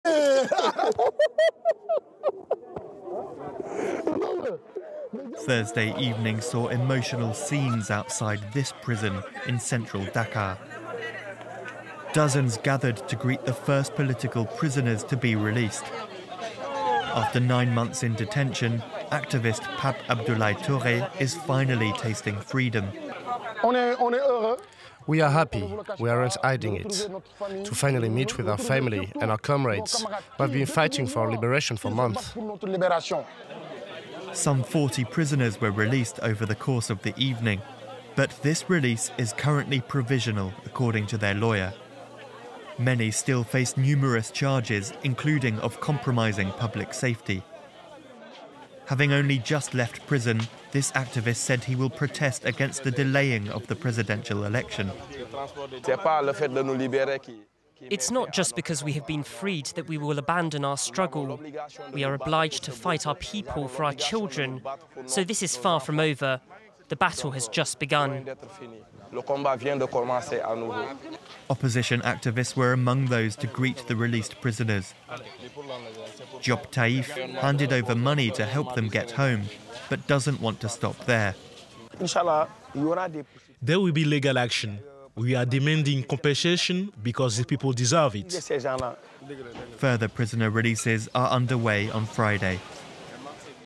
Senegal political crisis: More than 130 opponents released from prison • FRANCE 24 English
The Senegalese government has freed dozens of prisoners held since the outbreak of protests that have rocked the country since March 2021. Many have framed it as a sign of goodwill from the government, which is calling for a national dialogue after postponing the Presidential election initially scheduled for February. More prisoners could be released in the coming days, as our team on the ground reports.